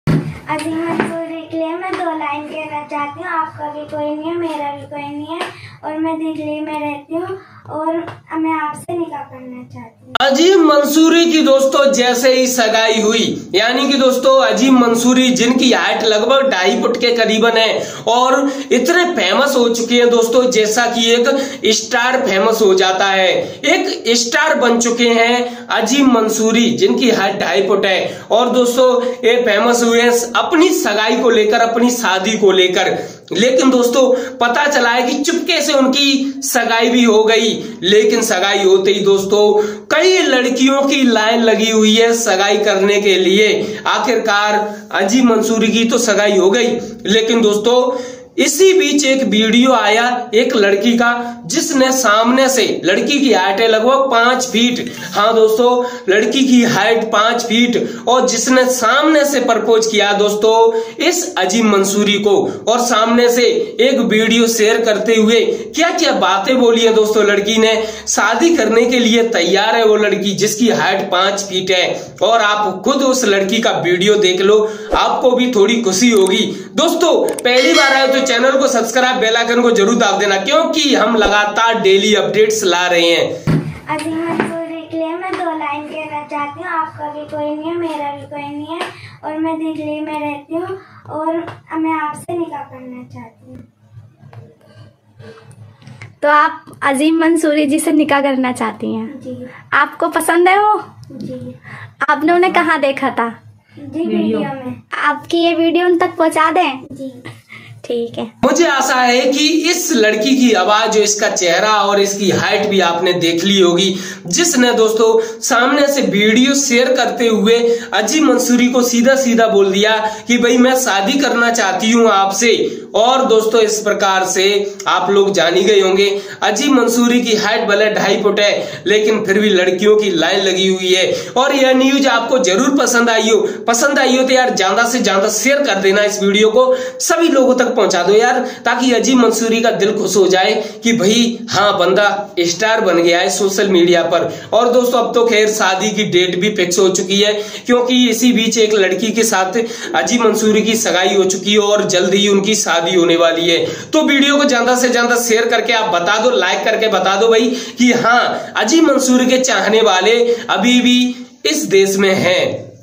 Azim Mansoori Wedding || अज़ीम मंसूरी के लिए लड़कियों की लाइन , 5 फ़ीट की लड़कियां भी सादी के लिए तैयार
NewsJankari

Follow us other Social Media Platform